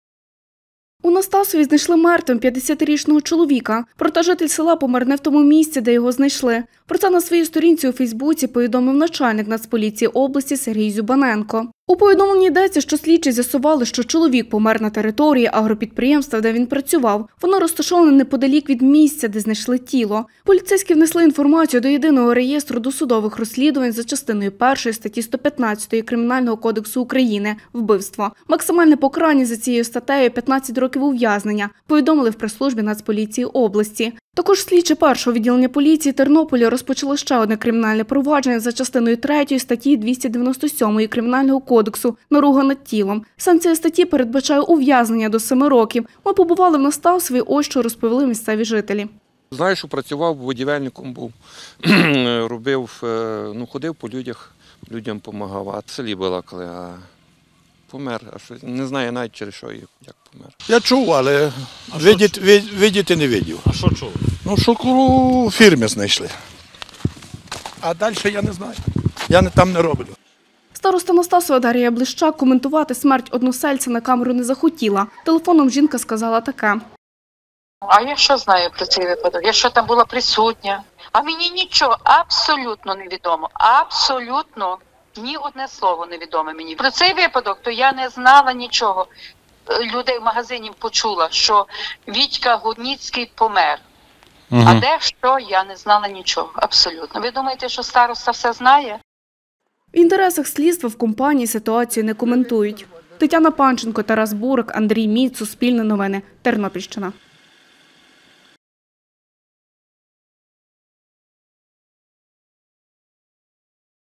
Знайшли мертвим на узбіччі
У селі Настасів Тернопільського району знайшли мертвим 50-річного чоловіка. За результатами розтину його смерть була ненасильницькою. Проте чоловік помер не в тому місці, де знайшли його тіло. Про це на своїй сторінці у фейсбуці повідомив начальник Нацполіції області Сергій Зюбаненко. Більше – у сюжеті.